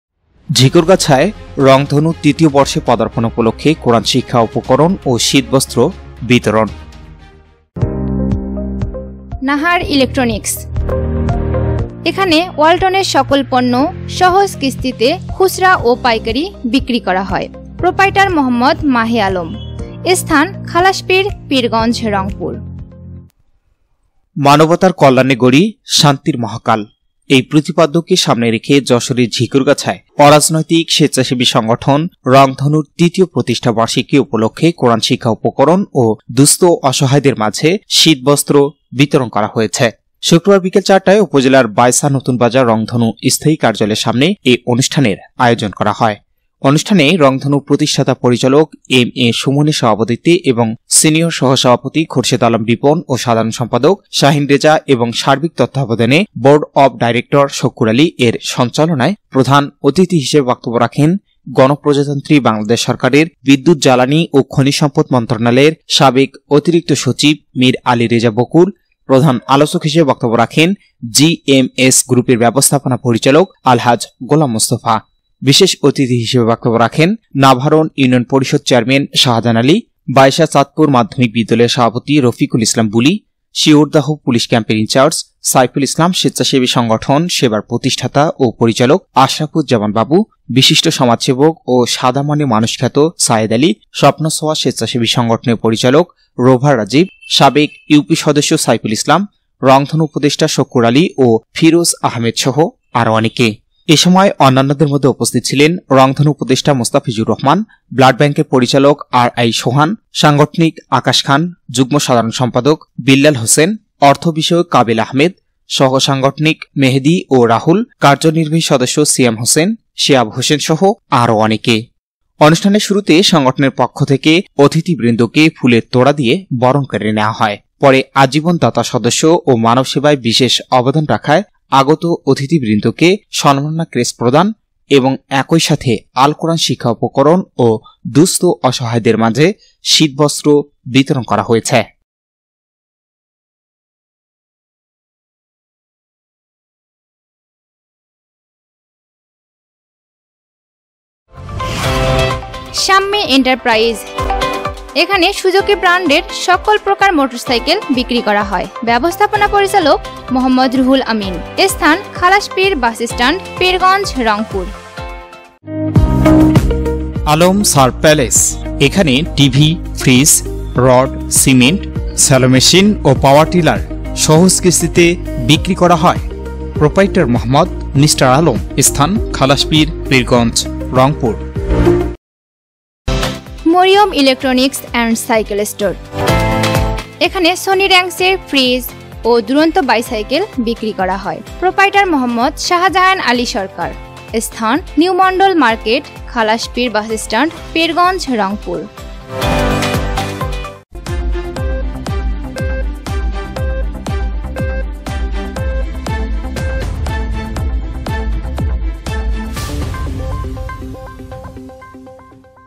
ঝিকরগাছায় রংধনু তৃতীয় বর্ষে পদার্পণ উপলক্ষে কোরআন শিক্ষা উপকরণ ও শীতবস্ত্র বিতরণ || ABN 24 NEWS
"মানবতার কল্যাণে গড়ি শান্তির মহাকাল"এই প্রতিপাদ্য কে সামনে রেখে যশোরের ঝিকরগাছায় অরাজনৈতিক সেচ্ছাসেবী সংগঠন রংধনু তৃতীয় প্রতিষ্ঠাবার্ষিকী উপলক্ষে কোরআন শিক্ষা উপকরণ ও দুঃস্থ-অসহায়দের মাঝে শীতবস্ত্র বিতরণ করা হয়েছে।